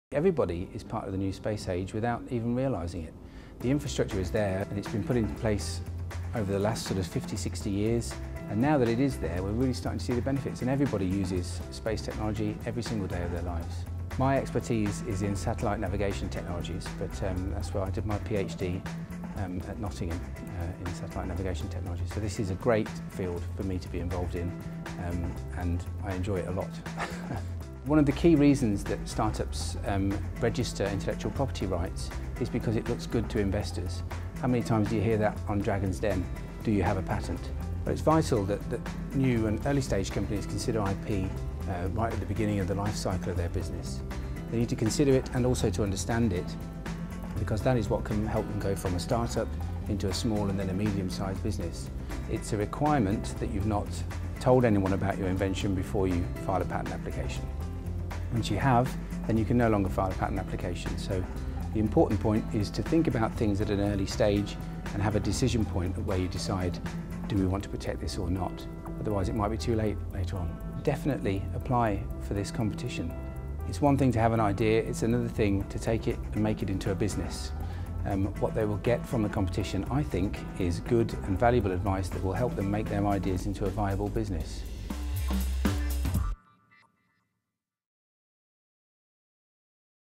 Protect your business idea
Stephen Blake from Marks & Clerk explains why patents count, and how the European Satellite Navigation Competition will help support your Intellectual Property.

Do you have a brilliant idea that uses satellite navigation technology? The European Satellite Navigation Competition (ESNC) is looking for individuals, teams or companies with ground-breaking ideas in applying positioning, navigation or timing technology, to rapidly accelerate their ideas into successful businesses.

Whether it's an app, software platform or new piece of technology, the ESNC can connect you with a world-class network and provide expert business mentoring with guidance in protecting Intellectual Property (IP), refining your business model and maximising investor appeal.